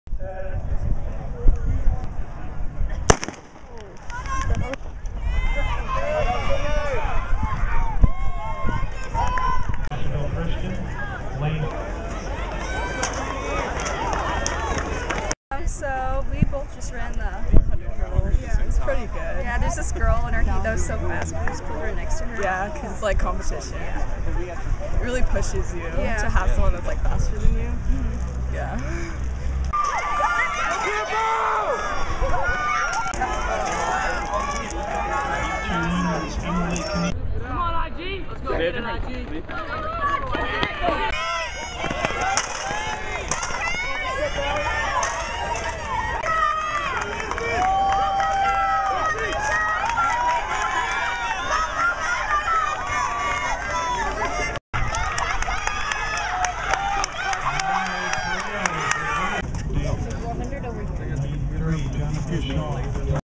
Halden Invitational 2015 part 2
At the Halden Invitational Track meet on Saturday, April 11, over 20 schools competed in a variety of track and field events. Here's some of the highlights of the hurdles runners. Produced by Emaan Riaz & Anna Gao.